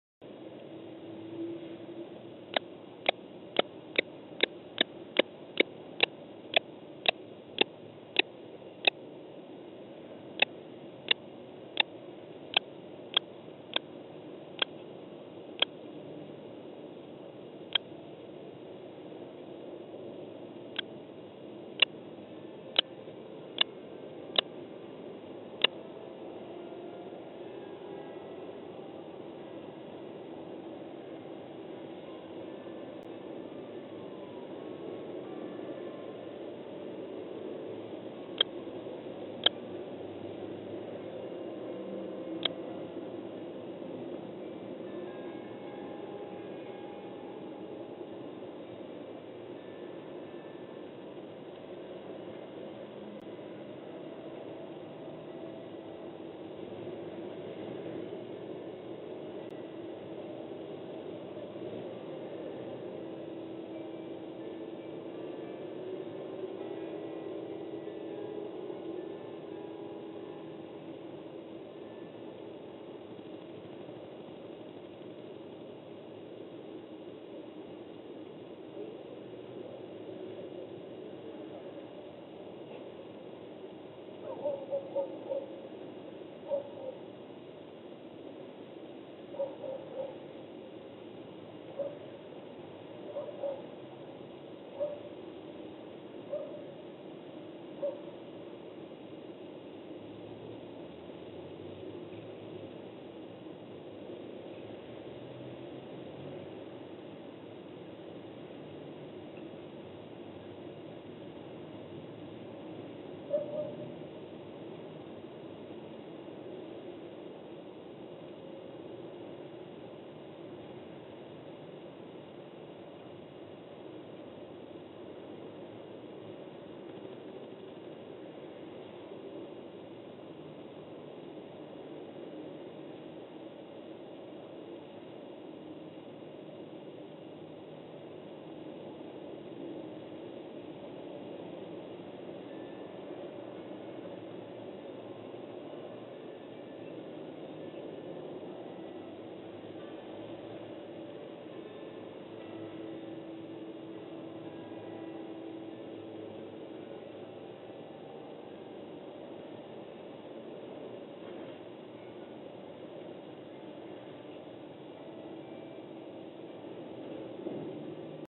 Odwiedziny pustułki po 47 dniach, 07.07.2014
Ostatni raz pustułka była obserwowana w otworze 20 maja.